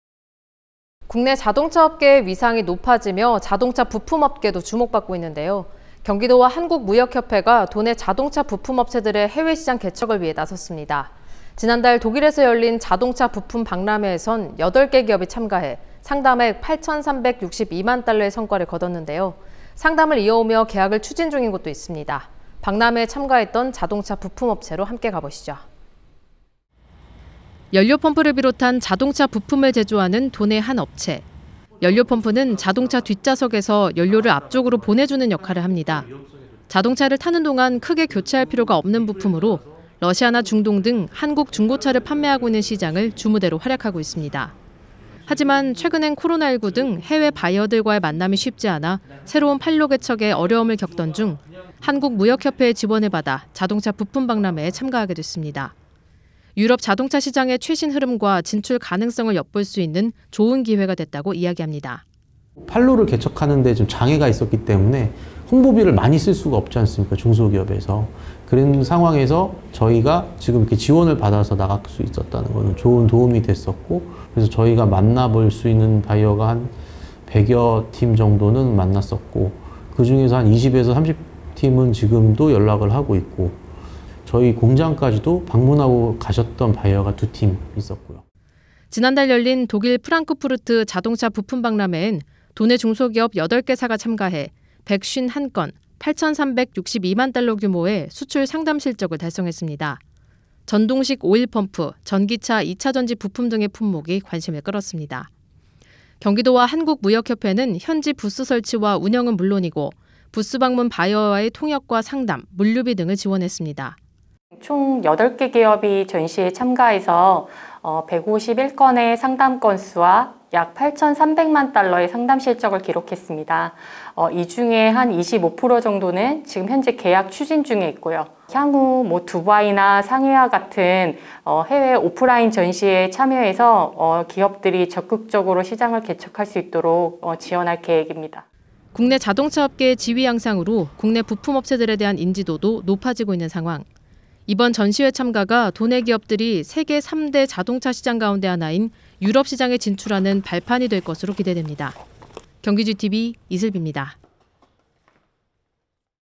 도내 8개 기업, 유럽 자동차 시장에 발판 마련
[앵커멘트]
국내 자동차 업계의 위상이 높아지며 국내 자동차 부품업계도 주목 받고 있는데요.
경기도와 한국무역협회가 도내 자동차 부품 업체들의 해외 시장 개척을 위해 나섰습니다.
지난달 독일에서 열린 자동차부품 박람회에선 8개 기업이 참가해 상담액 8천362만달러의 성과를 거뒀는데요.
상담을 이어오며 계약을 추진 중인 곳도 있습니다. 박람회에 참가했던 자동차 부품업체로 함께 가보시죠.

[리포트]
연료펌프를 비롯한 자동차 부품을 제조하는 도내 한 업체.
연료펌프는 자동차 뒷좌석에서 연료를 앞쪽으로 보내 주는 역할을 합니다.

자동차를 타는 동안 크게 교체할 필요가 없는 부품으로
러시아나 중동 등 한국 중고차를 판매하고 있는 시장을 주 무대로 활약하고 있습니다.

하지만 최근엔 코로나19 등 해외 바이어들과의 만남이 쉽지 않아
새로운 판로 개척에 어려움을 겪고 있던 중
한국무역협회의 지원을 받아 자동차부품박람회에 참가하게 됐습니다.

프랑크푸르트 자동차부품 박람회 참가로 유럽 자동차 시장의 최신 흐름과
진출 가능성을 엿볼 수 있는 좋은 기회가 됐다고 이야기 합니다.

[인터뷰] 유재민 / (주)유창하이텍 자동차부품팀 이사
(코로나19로) 판로를 개척하는 데 장애가 있었기 때문에 홍보비를 많이 쓸 수 없지 않습니까 중소기업에서.
그런 상황에서 지원 받아서 나갈 수 있었다는 게 도움이 됐고요. (박람회에서)만나볼 수 있는 바이어가 100여 명 정도 만났고요.
그 중 2~30명은 지금도 연락하고 있고 공장까지 방문하고 간 바이어가 2~3분 있었고..

지난달 열린 독일 프랑크푸르트 자동차 부품 박람회엔 도내 중소기업 8개 사가 참가해
151건, 8천 362만 달러 규모의 수출 상담 실적을 달성했습니다.

전동식 오일펌프, 전기차 2차전지 부품 등의 품목이 관심을 끌었습니다.

경기도와 한국무역협회는 현지 부스 설치와 운영은 물론이고
부스 방문 바이어와의 통역과 상담, 물류비 등을 지원했습니다.

[인터뷰] 심혜정 / 한국무역협회 경기본부 팀장
총 8개 기업이 전시회에 참가해서 151건의 상담건수와 약 8300만 달러의 상담실적을 기록했습니다.
이 중 25% 정도는 현재 계약 추진 중에 있고요.
향후 두바이나 상해와 같은 해외 오프라인 전시에 참여해서 기업들이 적극적으로 시장개척을 할 수 있도록 지원할 계획입니다.

국내 자동차업계의 지위 향상으로 국내 부품업체들에 대한 인지도도 높아지고 있는 상황.
이번 전시회 참가가 도내 기업들이 세계 3대 자동차 시장 가운데 하나인
유럽 시장에 진출하는 발판이 될 것으로 기대됩니다.

[자막]

1. 도내 자동차 부품 제조 업체

2. 교체 주기 긴 편으로 한국 중고차를 판매하는 해외 시장을 무대로 활약

3. 새로운 판로 개척을 위해 박람회 참가

4. 유럽 자동차 시장 최신 흐름과 진출 가능성 엿보아

5. [인터뷰] 유재민 / ㈜유창하이텍 자동차부품팀 이사

6. 도내 중소기업 8개사 참가, 8천 362만 달러 수출 상담 실적 달성

7. 오일펌프, 전기차 2차전지 부품 등이 인기

8. 경기도-한국무역협회가 통역과 상담, 물류비 지원

9. [인터뷰] 심혜정 / 한국무역협회 경기본부 팀장

10. 전시회 참가로 해외 시장 진출에 발판 마련

11.  영상취재: 이효진 이민준   영상편집: 윤지성